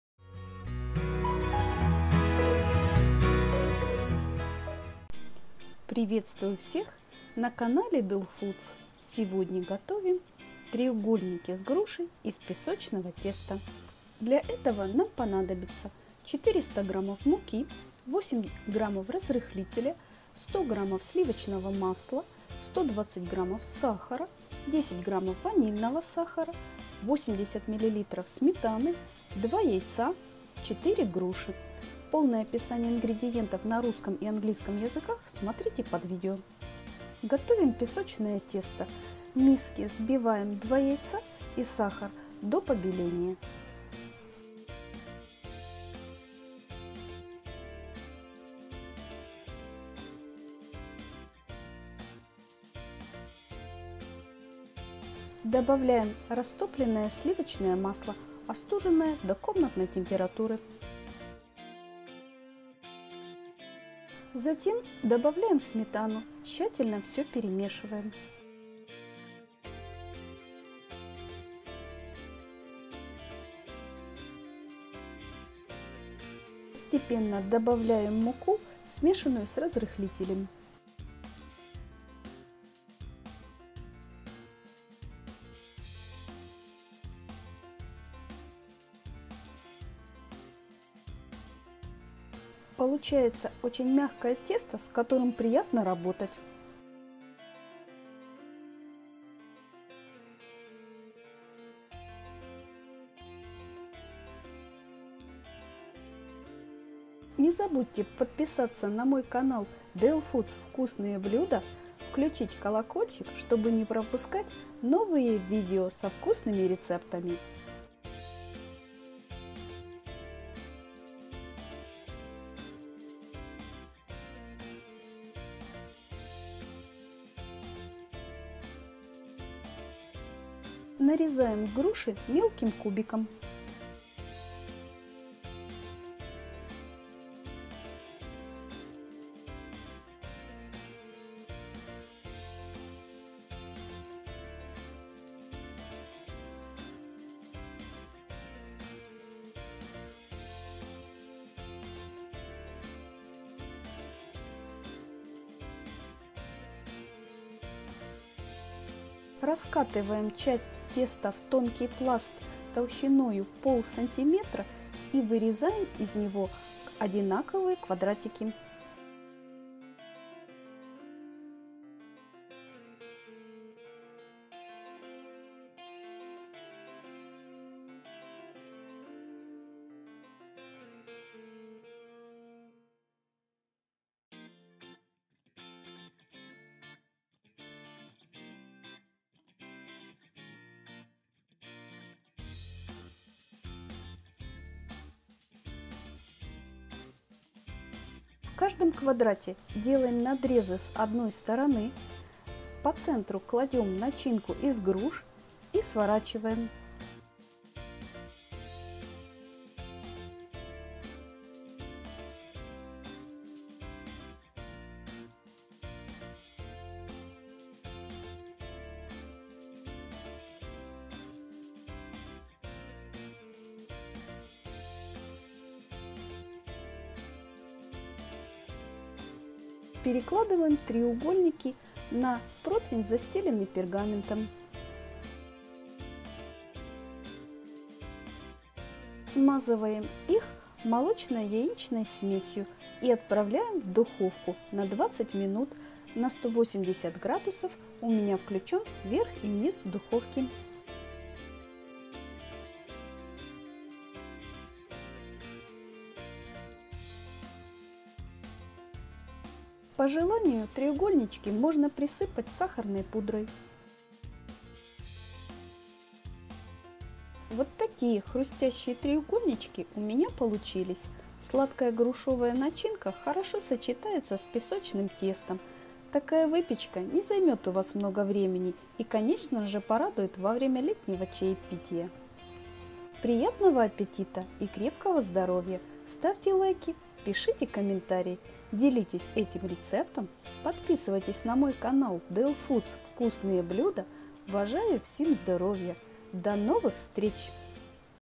Треугольники с грушей 🍐 из песочного теста 😋! СУПЕР вкус!
Приготовьте эти простые треугольники с грушей 🍐 из песочного теста! Вкусная выпечка из простых продуктов быстрого приготовления! Супервкусные треугольники к летнему чаепитию!

Сметана                                                 80 мл
Яйца                                                           2
Груши                                                         4

⭐Flour                                                 400g
Sour cream                                          80 ml
Eggs                                                          2
Pears                                                        4